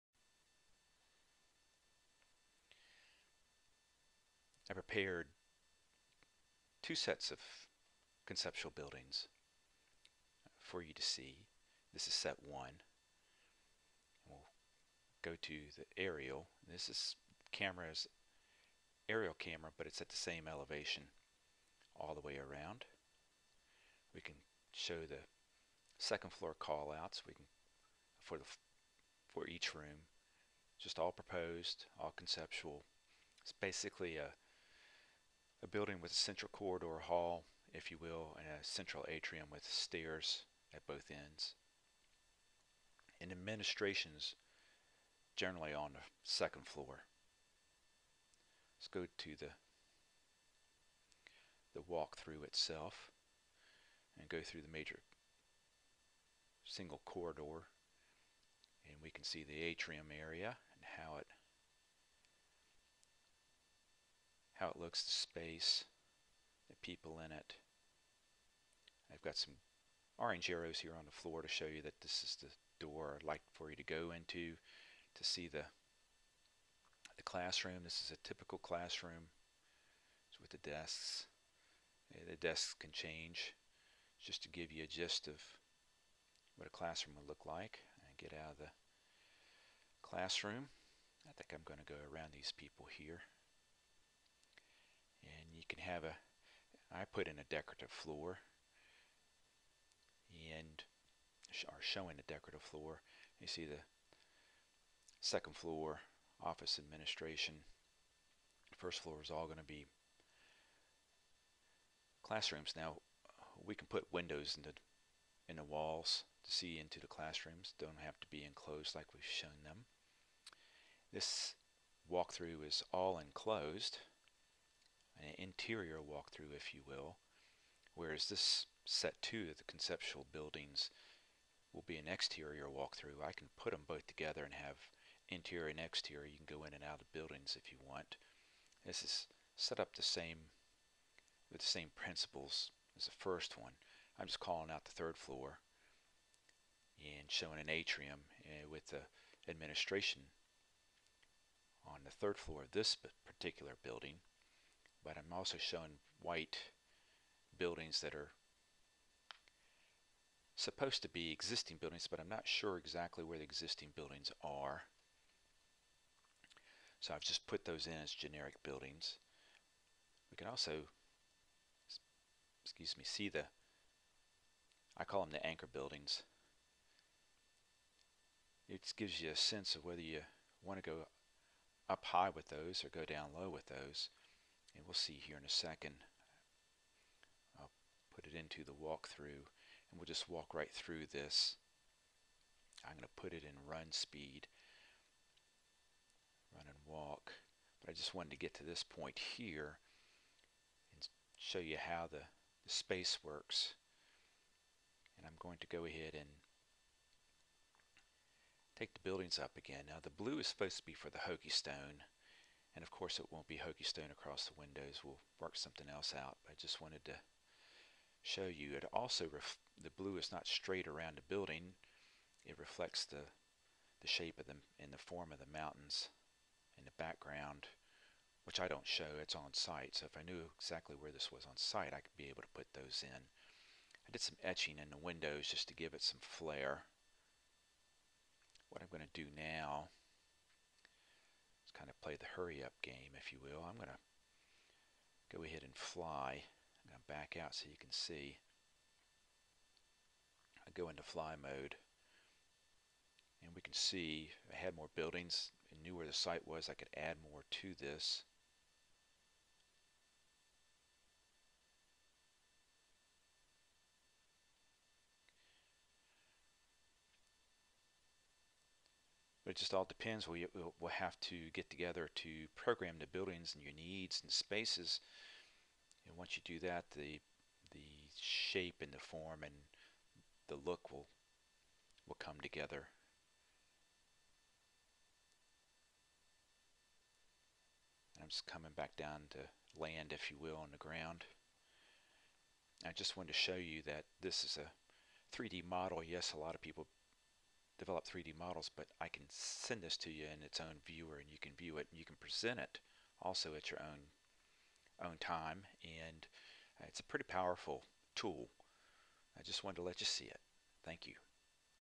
vt concept buildings
Built 3d models and prepared walk thru, captured walk thru and re-coded it from 1.7 gig to 80 megs... Voice over is usually written in advance but this VO is on the fly... and yes it is still at 60 fps.